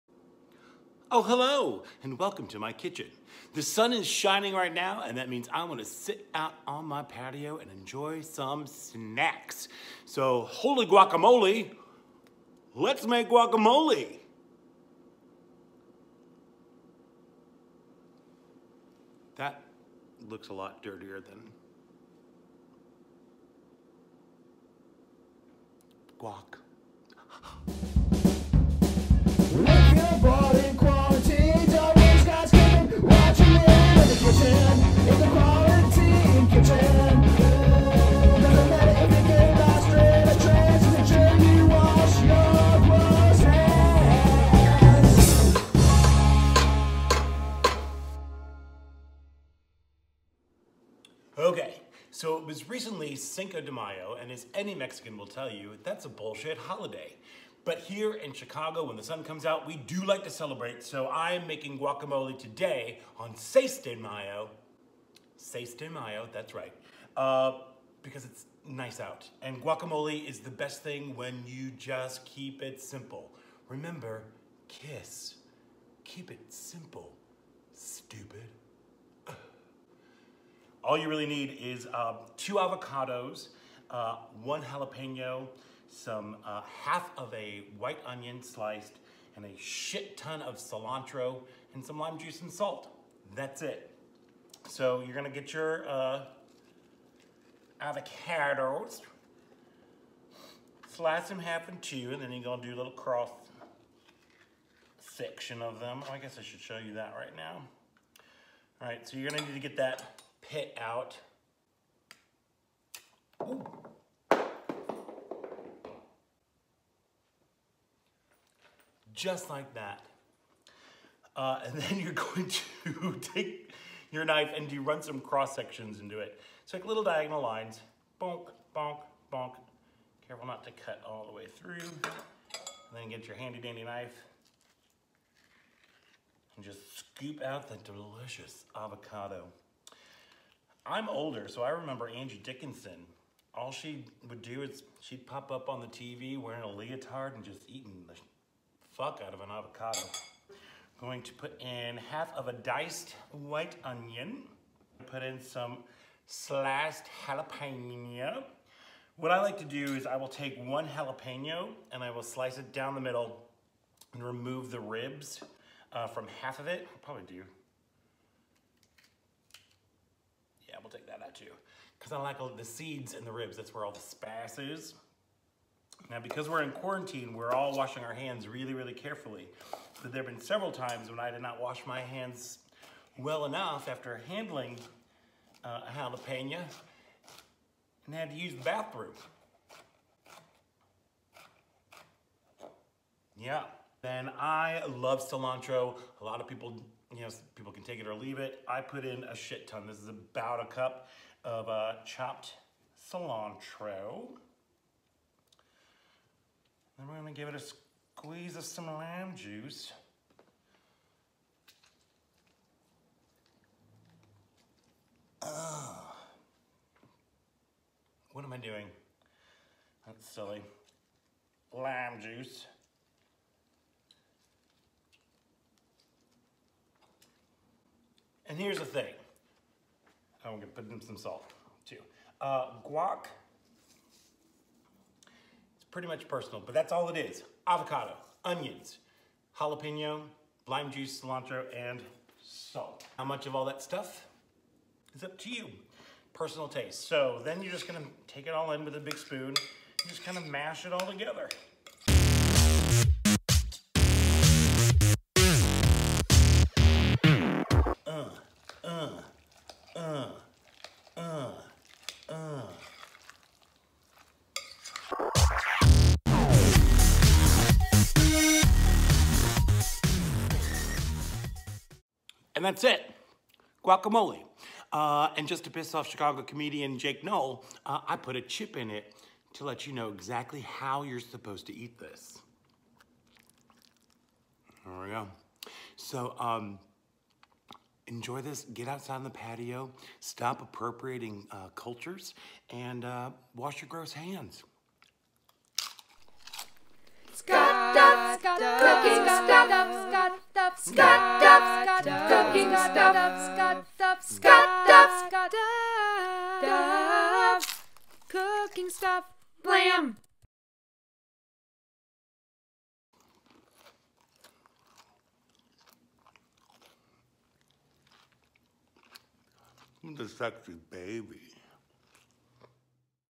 Scott Duff Cooking Stuff: Quarantine Kitchen - Guacamole
Holy Guacamole, it's guacamole! I've had some fancy schmancy guac in my day, but the best is the simple stuff: avocado, onion, jalapeño, cilantro, and lime juice. Great with chips, in a burrito, or slap some on a burger.